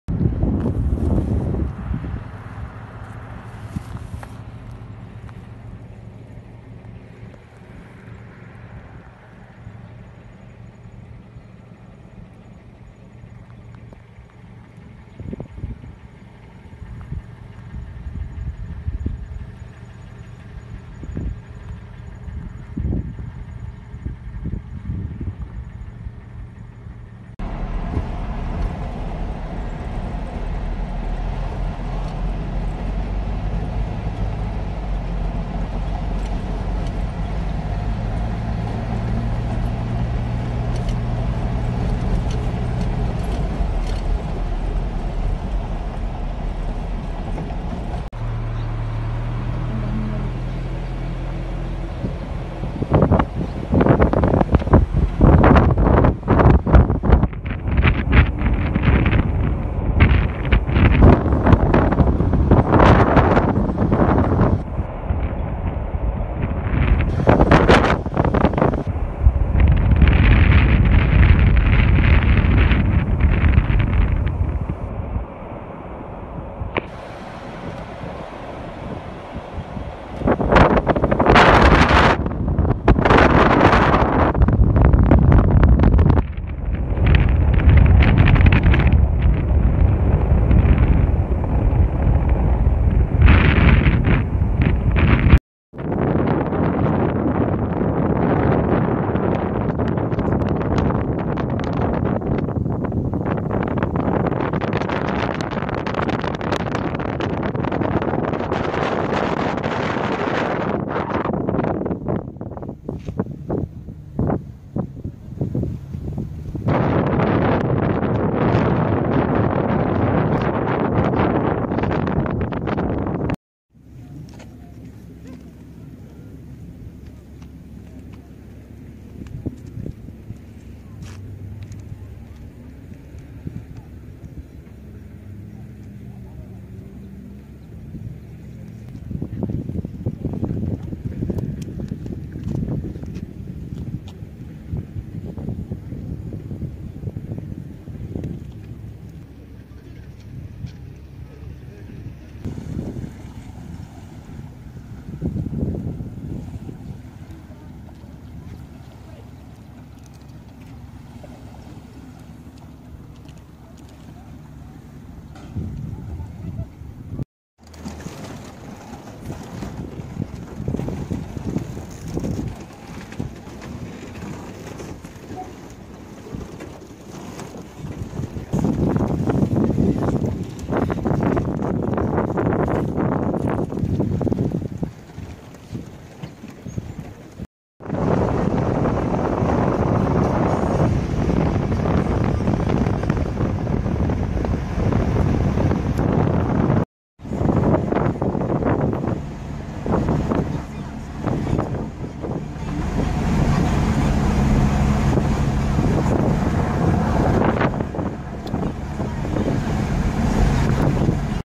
Piru Lake Ventura county
Lake Piru Great place to spend time is a located in   Los Padres National Forest  Ventura county California